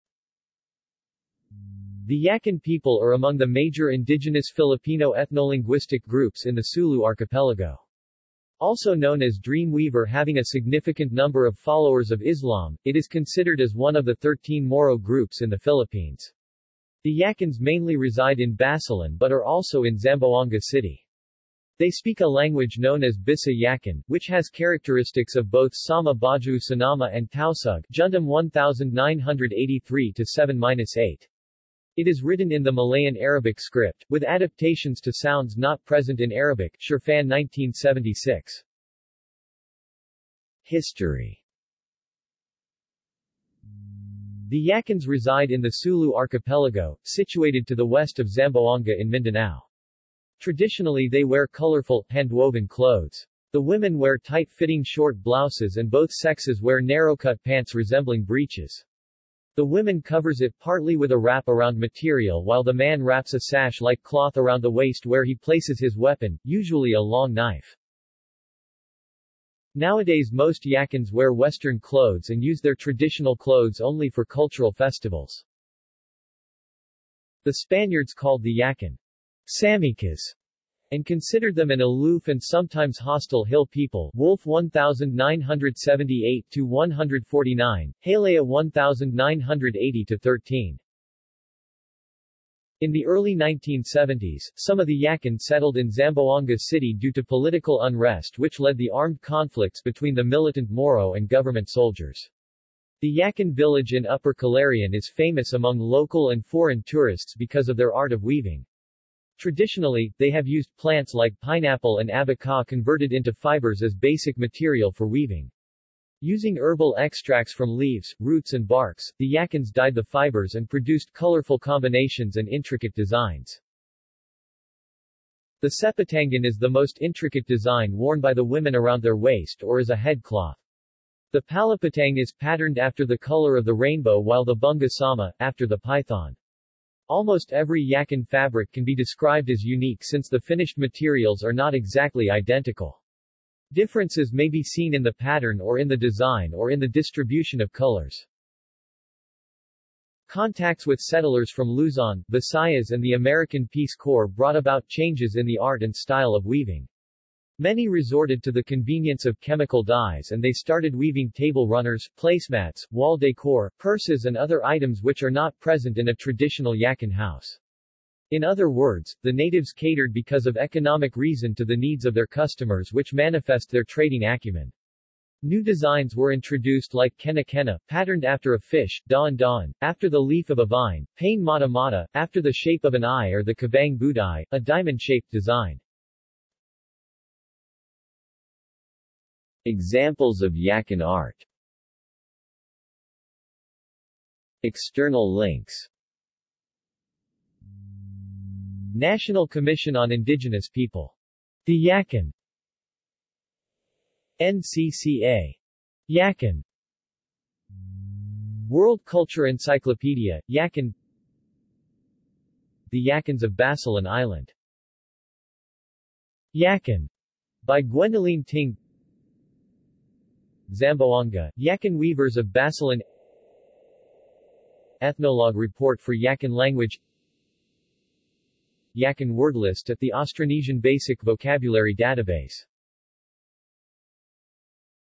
Yakan | Wikipedia audio article
This is an audio version of the Wikipedia Article:
Yakan

SUMMARY
The Yakan people are among the major indigenous Filipino ethnolinguistic groups in the Sulu Archipelago. Also known as dream weaver having a significant number of followers of Islam, it is considered as one of the 13 Moro groups in the Philippines. The Yakans mainly reside in Basilan but are also in Zamboanga City. They speak a language known as Bissa Yakan, which has characteristics of both Sama-Bajau Sinama and Tausug (Jundam 1983: 7-8). It is written in the Malayan Arabic script, with adaptations to sounds not present in Arabic (Sherfan 1976).